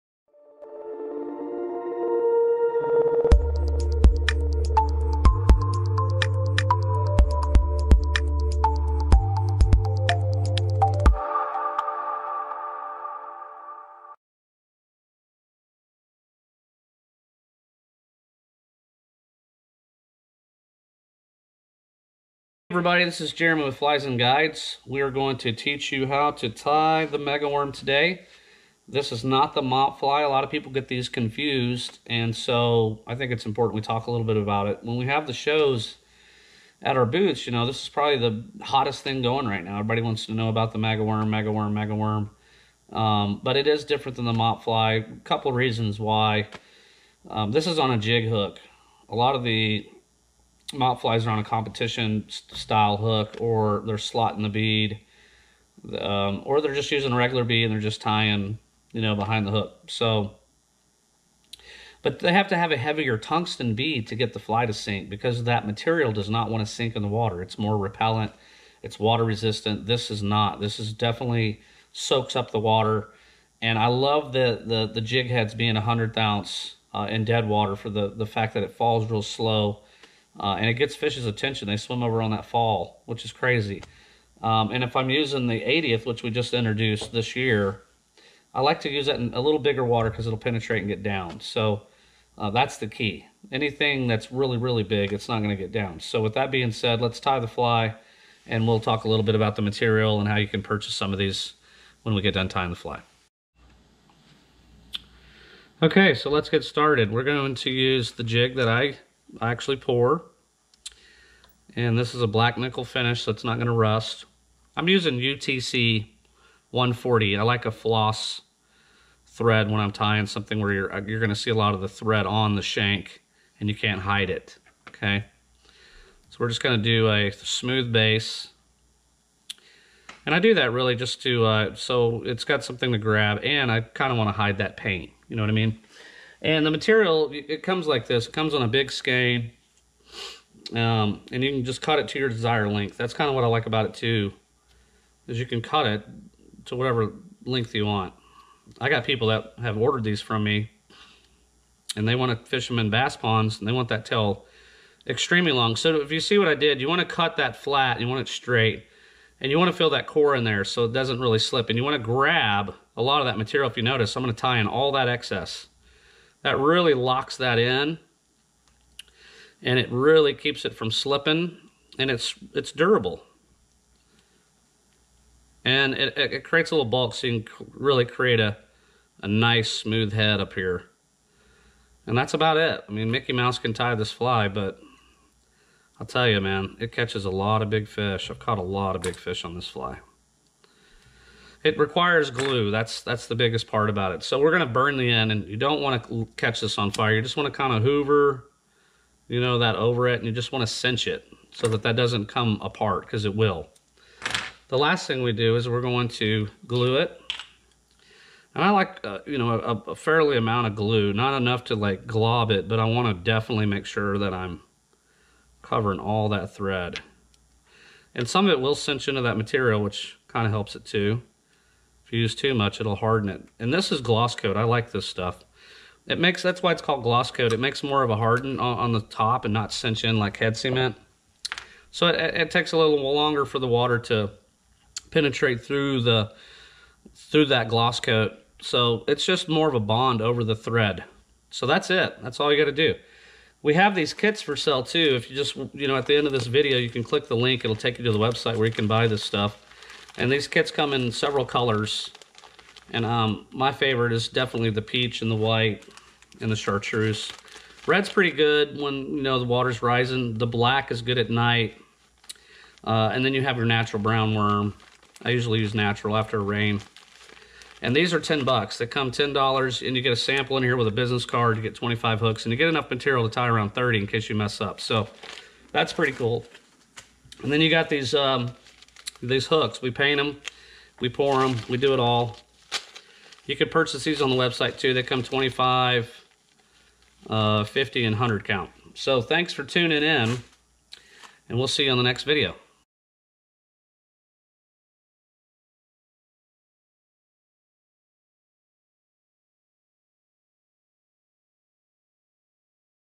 How to Tie | MegaWorm
Despite the ease of tying this fly, it is one of the most asked about flies that we sell and fish.  This is the way we tie the pattern and believe doing it in this manner will reduce the fly coming apart when fishing and will give the pattern that it needs to be one of the most productive flies in your arsenal.